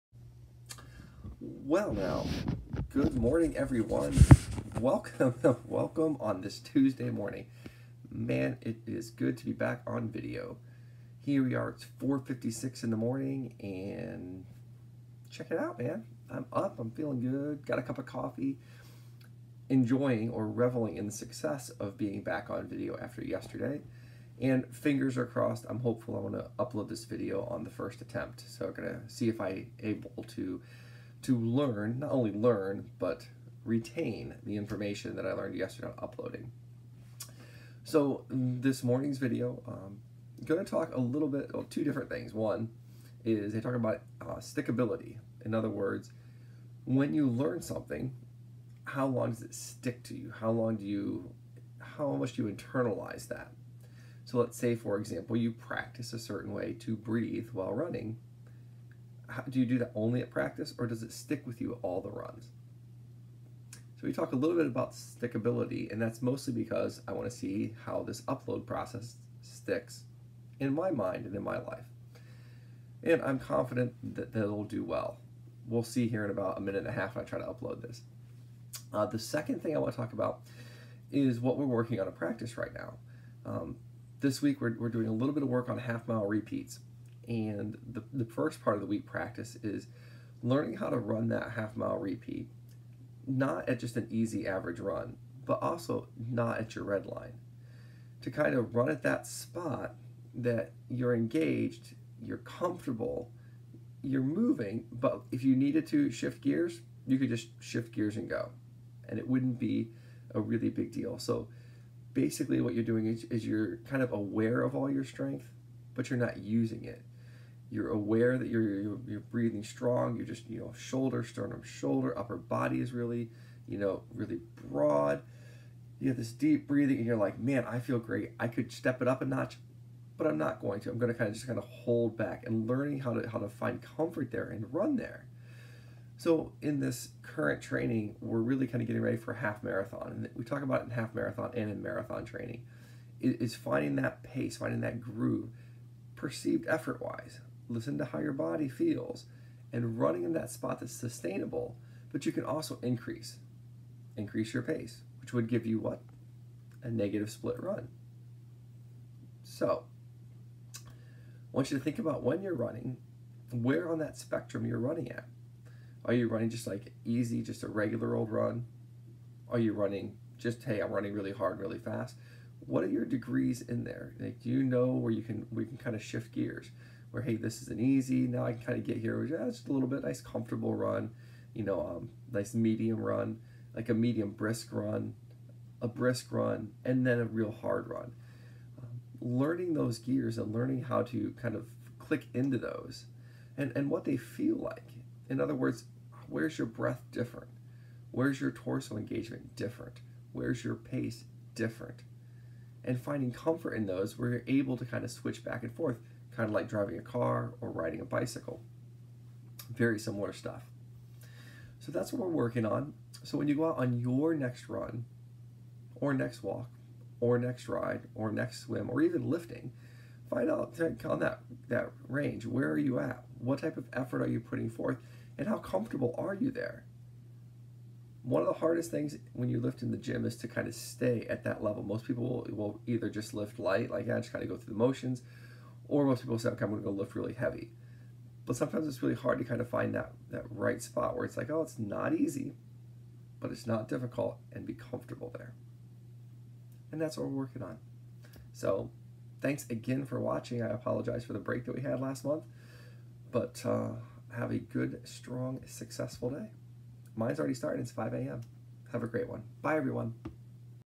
11/27/18. Leapfrog Video of the Day.
Talking about retention of skill & management of effort.